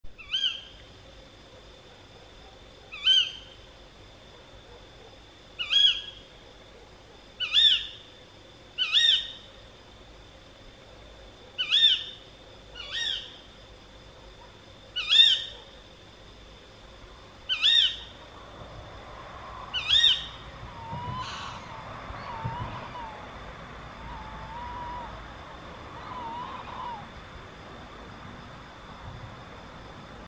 Female Tawny Owl Calling in the Night
A female tawny owl calling in the night near Gythio in the Peloponnese, Greece.